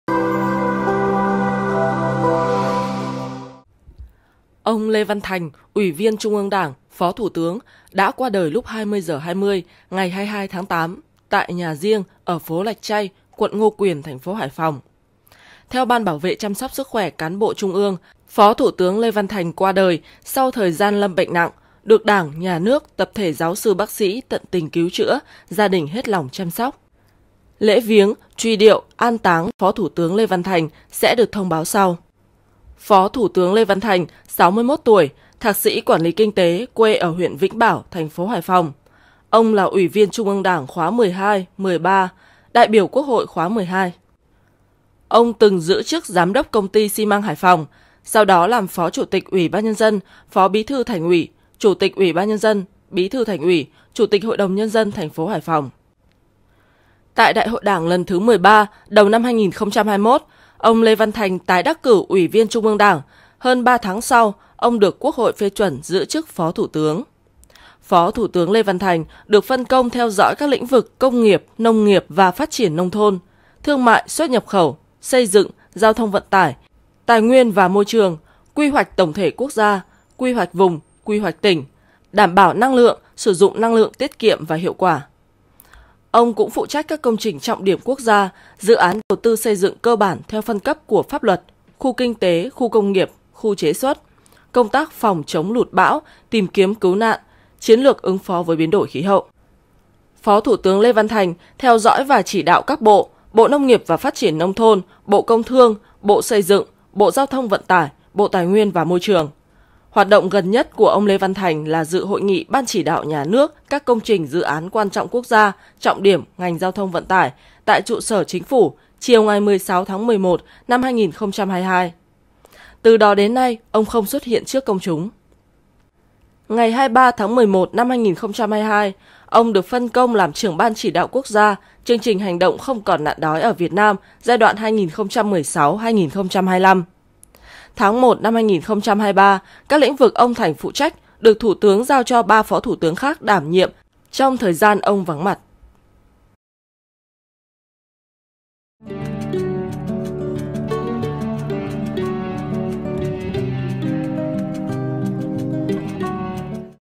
Phó Thủ Tướng Lê Văn Thành Qua Đời Tại Nhà Riêng Sau Thời Gian Lâm Bệnh Nặng | SKĐS
SKĐS | Theo Ban Bảo vệ chăm sóc sức khỏe cán bộ Trung ương, Phó thủ tướng Lê Văn Thành qua đời sau thời gian lâm bệnh nặng, được Đảng, Nhà nước, tập thể giáo sư, bác sĩ tận tình cứu chữa, gia đình hết lòng chăm sóc.. Ông Lê Văn Thành, Ủy viên Trung ương Đảng, Phó thủ tướng, qua đời lúc 20h20 ngày 22/8 tại nhà riêng ở phố Lạch Tray, quận Ngô Quyền, TP Hải Phòng.